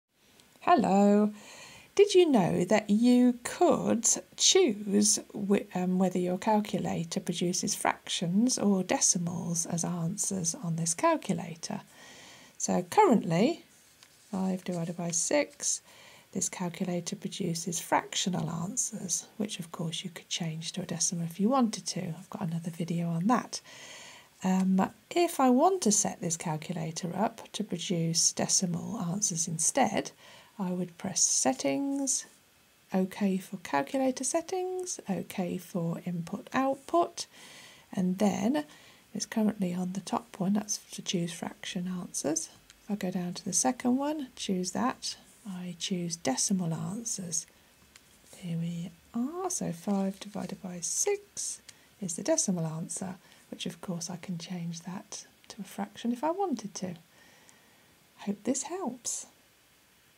Choose Fractions or Decimals as Your Default Output to Maths Calculations on the Casio fx-991CW
This video shows you how to change the default settings on your Casio fx-991CW to give an answer that will always be either a decimal or a fraction.